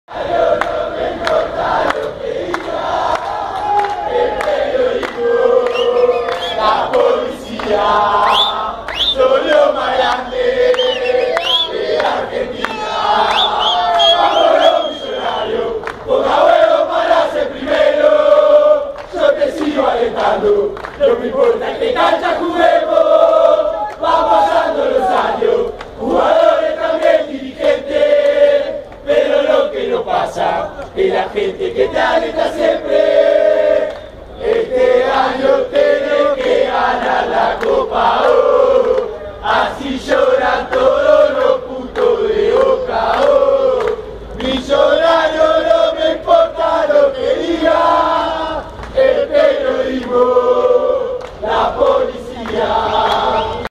Torcedores do River cantam na porta do hotel para incentivar o time
River Plate faz o segundo confronto no Mineirão contra o Cruzeiro, na noite desta quarta-feira, 27 de maio. O time mineiro tem a vantagem do empate sem gols por ter vencido a primeira partida na casa do adversário.